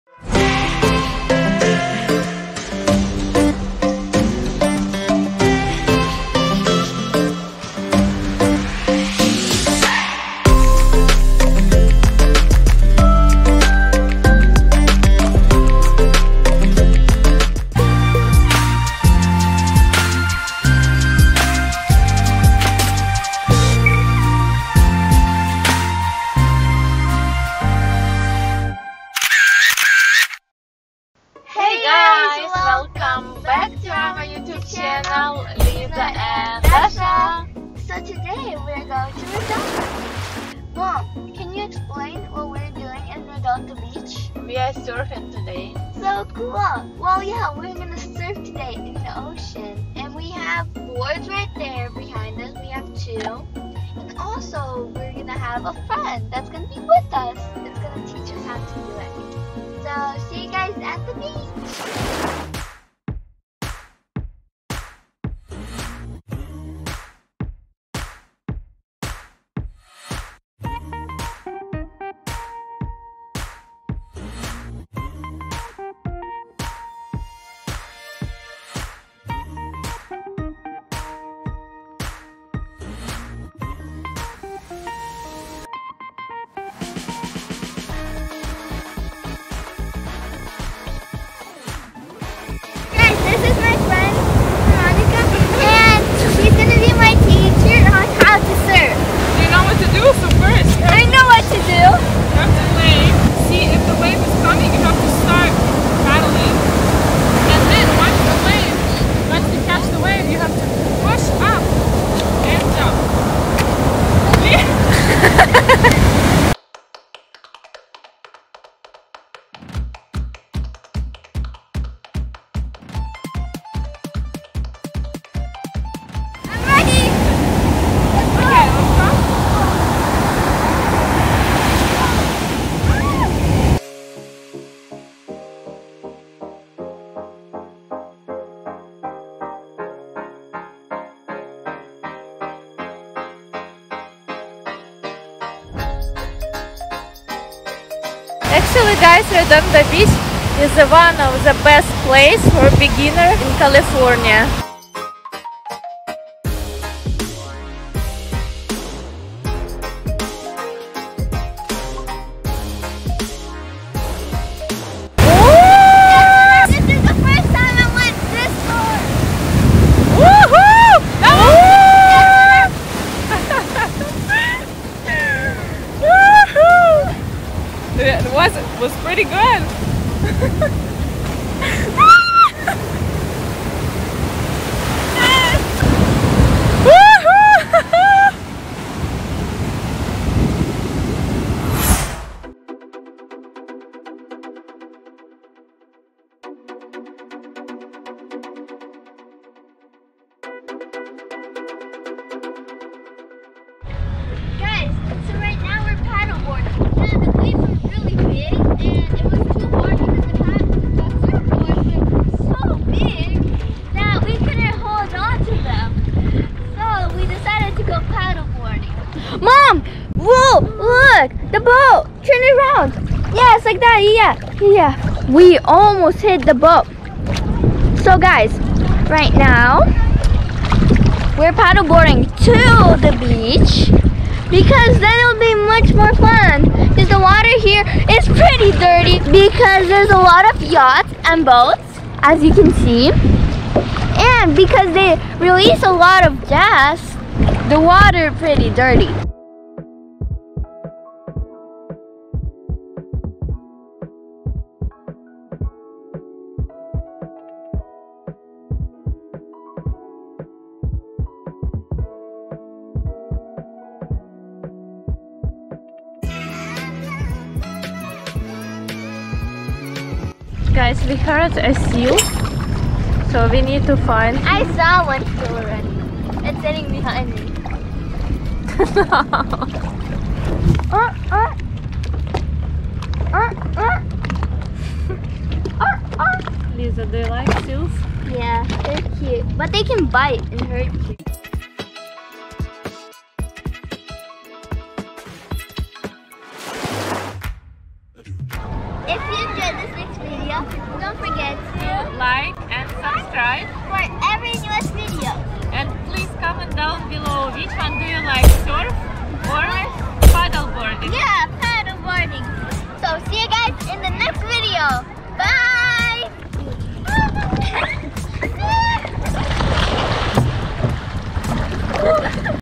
PADDLE BOARD VS SERF
Hello, In this video we did something really awesome which is paddle board VS surf. We went to Redonda beach to surf and Marina Del Rey ( mothers beach) if you want to find out why we did to activities then watch the video to find out!